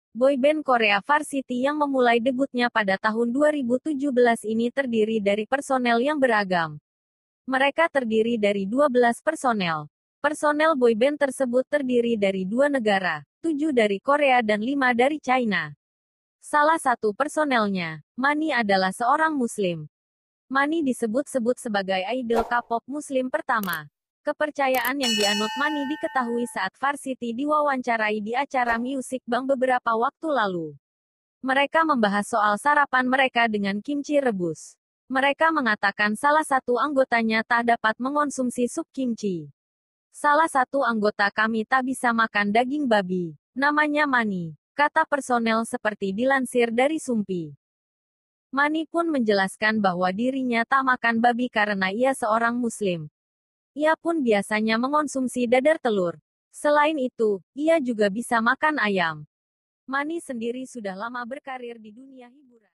Apa Kabar Manny VARSITY, Idol K-Pop Pertama yang Beragama Islam
Boyband Korea VARSITY yang memulai debutnya pada tahun 2017 ini terdiri dari personel yang beragam. Mereka terdiri dari 12 personel. Personel boyband tersebut terdiri dari dua negara, 7 dari Korea dan 5 dari China. Salah satu personelnya, Manny adalah seorang Muslim.

Manny disebut-sebut sebagai idol K-Pop muslim pertama. Kepercayaan yang dianut Manny diketahui saat VARSITY diwawancarai di acara Music Bank beberapa waktu lalu. Mereka membahas soal sarapan mereka dengan kimchi rebus. Mereka mengatakan salah satu anggotanya tak dapat mengonsumsi sup kimchi.

"Salah satu anggota kami tak bisa makan daging babi, namanya Manny," kata personel seperti dilansir dari Soompi.

Manny pun menjelaskan bahwa dirinya tak makan babi karena ia seorang muslim. Ia pun biasanya mengonsumsi dadar telur. Selain itu, ia juga bisa makan ayam.

Manny sendiri sudah lama berkarier di dunia hiburan. Manny dikenal sebagai artis cilik. Ia memulai debutnya sebagai artis di usia 3 tahun. Ia juga memulai kariernya sebagai model pada usia 10 tahun. Pria kelahiran China ini merupakan keturunan Uighur. Makanan favoritnya adalah ayam, daging domba dan sapi.

Ia pun dikenal sangat jago menari. Hobinya adalah menari. tidur, menyanyi dan bermain basket. Pria kelahiran 17 November 2001 ini sangat mengidolakan Michael Jackson dan Chris Brown.

Manny sendiri sempat menghebohkan penggemarnya saat ia mengunggah fotonya saat beribadah di salah satu masjid. Ia mengunggahnya di Twitter.

Pemilik nama asli Xiao Dongcheng ini juga pernah mengucapkan selamat menjalani ibadah di bulan Ramadhan pada tahun 2019 lalu melalui Instagram miliknya, @manny_arab.

Hanya saja, beberapa waktu belajangan ini, Manny dan grupnya memang jarang eksis di dunia hiburan. Belum lama ini, Manny mempromosikan akun Tiktok milknya di Instagram.

😁," tulis Manny.

Postingan itu langsung ramai dengan komentar penggemarnya. Banyak penggemar yang mengucapkan selamat menjalankan ibadah puasa di bulan Ramadhan untuk Manny.

"RAMADAN MUBARAK, Manny!," tulis netizen dengan akun amandad0722

"@manny_arab happy Ramadan kareem," tulis netizen dengan akun myg_shooky.

Beberapa juga menuangkan kerinduannya dengan menyapa Manny. Mereka antusias melihat postingan Manny yang asyik bermain TikTok.

Kehadiran Manny sebagai idol Muslim pertama mendapat sambutan positif dari penggemar musik K-Pop. Mereka senang karena K-Pop terus berkembang dan menghadirkan banyak penggemar seluruh dunia.